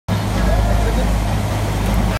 Barracuda Attack - Off St. Kitts / Nevis
We caught this great barracuda off the coast of St. Kitts and Nevis. The guide didn't want me to hold the fish, but everyone wanted me to hold it for a picture. When I did, the fish saw his reflection in my sunglasses and then reached back, grabbed the glasses and tossed them right into the ocean. yes, the fish was returned to the sea and he is now wearing a very nice pair of glasses.